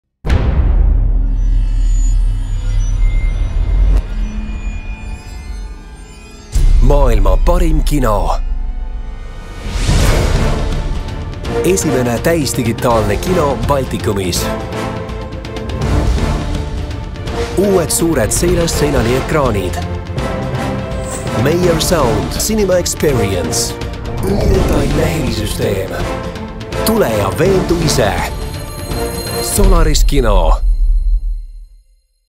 Maailma Parim Kino - Solaris Kino
Solaris Kino on maailma parim kino.

Milline on maailma kõige parem kino? Meie usume, et maailma parim kino on siinsamas Tallinnas ja selle nimi on Solaris Kino. Teeme seda mitte endale, vaid Sulle ja Sinu sõpradele. Kõigile. kes oskavad ja tahavad kinokunsti nautida.

Meie missioon on olla kvaliteedimärk ja teerajaja nii Eestis kui kogu lähiregioonis. Pärast suuri tehnilisi arendustöid on Solaris Kino regiooni esimene täisdigitaalne kinokeskus, millega suudavad konkureerida vaid üksikud parimad kinod maailmas. Pingutame iga päev, et pakkuda Solaris Kino külalistele maailma parimat kinoelamust. Uuendused  on esiteks toonud kõikidesse Solaris Kino saalidesse maailmaklassi Dolby 3D täisdigitaalse kinosüsteemi koos teravaima graafika ja rabavate värvidega. Selleks paigaldasime saalidesse uusimad suure valgusjöuga Barco ja NEC teise põlvkonna digikino projektorid. Teiseks on külastajate ettepanekutest lähtuvalt kõikides kinosaalides senisest kohati isegi kaks korda suuremad seinast seinani ulatuvad uued ekraanid. Kolmandaks on esimesena Euroopas kasutusele võetud Meyer Sound Laboratories ülidetailne Cinema Experience helisüsteem nüüd Dolby Surround 7.1 valmidusega.

Kokkuvõttes on Solaris Kino nüüd täielikult varustatud maailma parima kinotehnikaga, mis toob filmid Eesti vaatajateni täpselt sellises kvaliteedis, nagu need on loodud.

Solarise keskuse teisel ja kolmandal korrusel asuvas Solaris Kinos on kokku 7 tipptasemel saali, teiste seas Eesti suurim kaasaegne kinosaal.

Hooliv - Sõbralik - Kodumaine

Sinu Solaris Kino